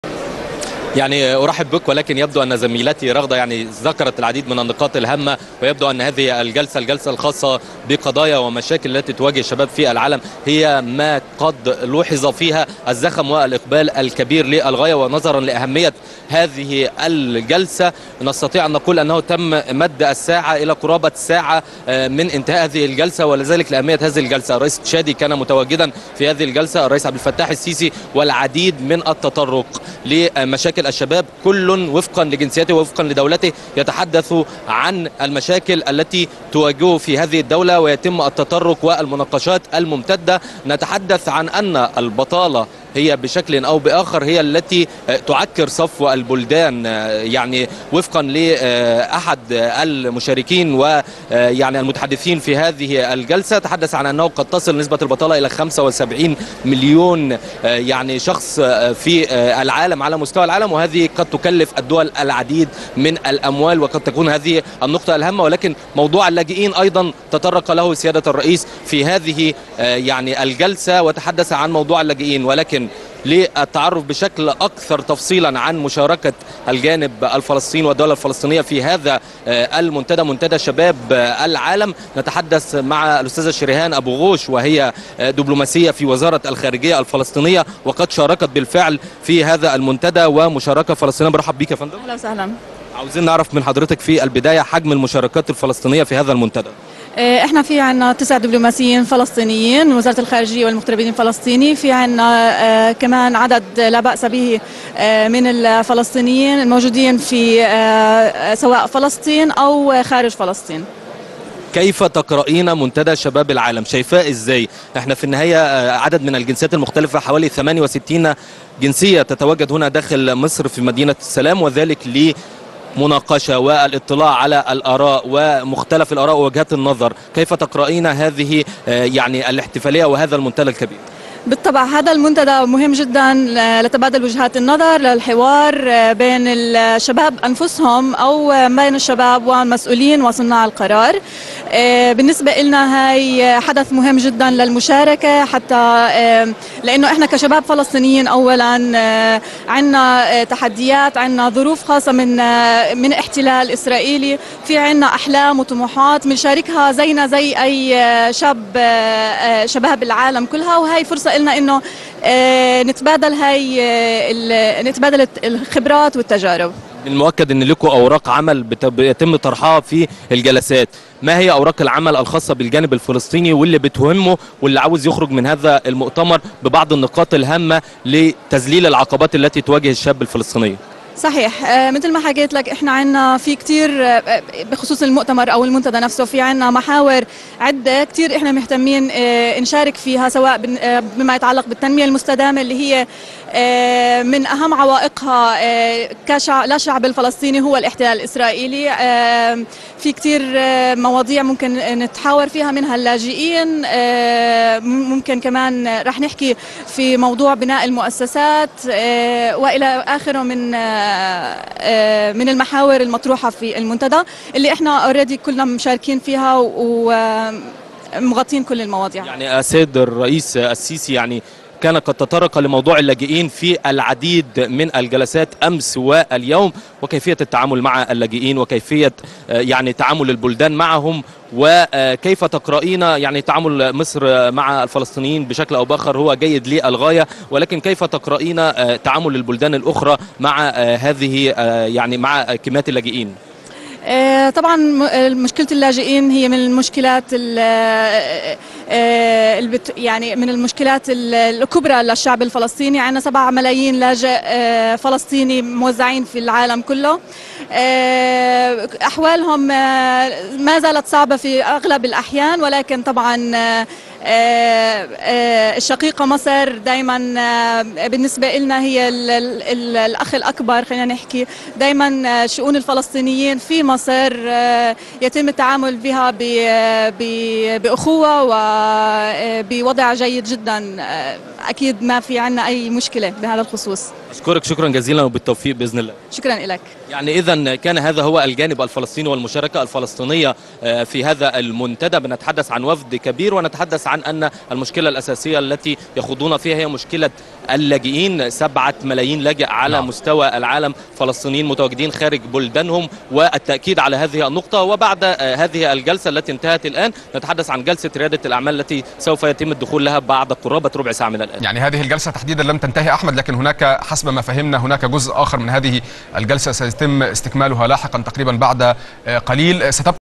لقاء لـ أون لايف مع شريهان أبو غوش الدبلوماسية بالخارجية الفلسطينية على هامش منتدى شباب العالم
الموقع الرسمي للقناة  ..